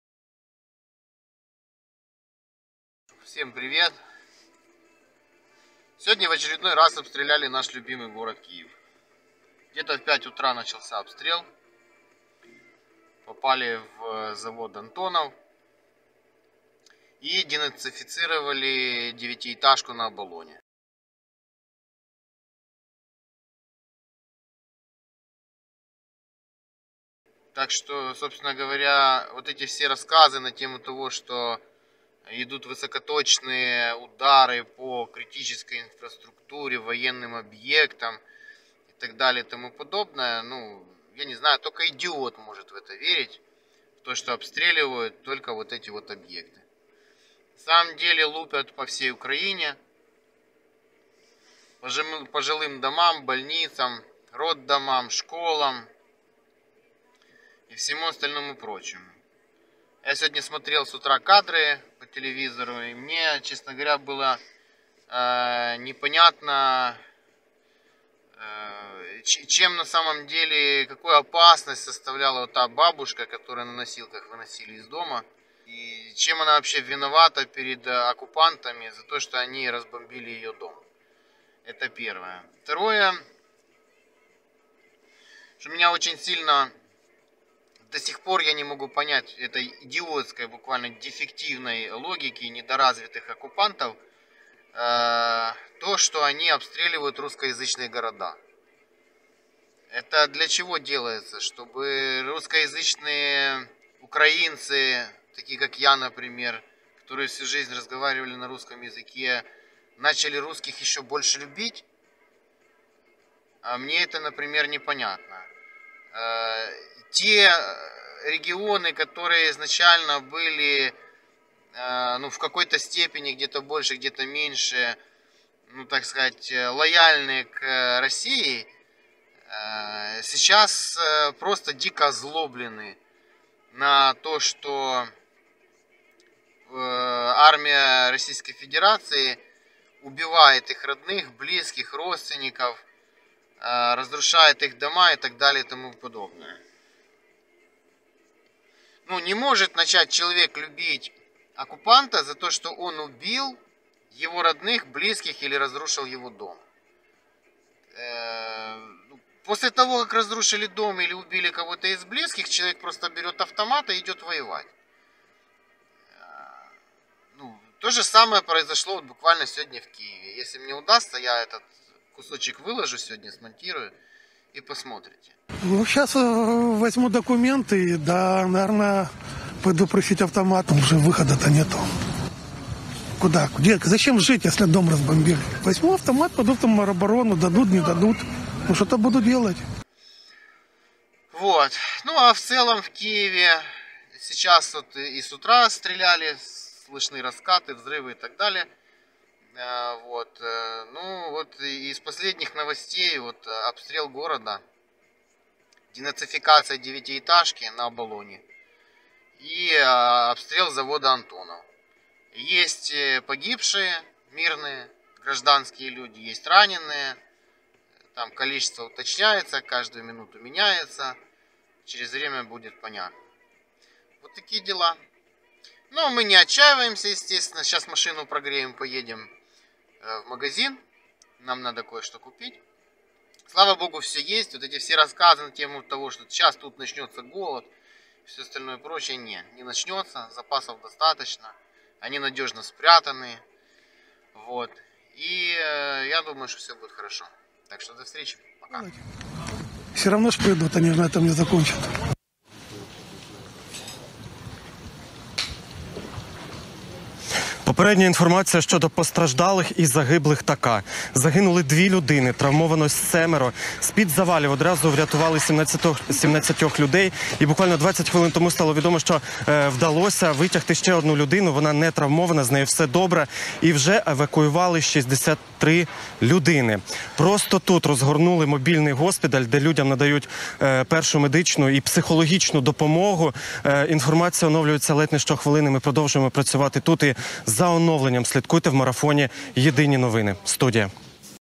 ОБСТРЕЛ КИЕВА СЕГОДНЯ
В очередной раз, сегодня, был обстрелян город Киев. "Денацифицирована" девятиэтажка на Оболони, пострадали и были убиты гражданские мирные люди. Война в Украине продолжается, оккупанты продолжают убивать невинных мирных людей и разрушать их дома.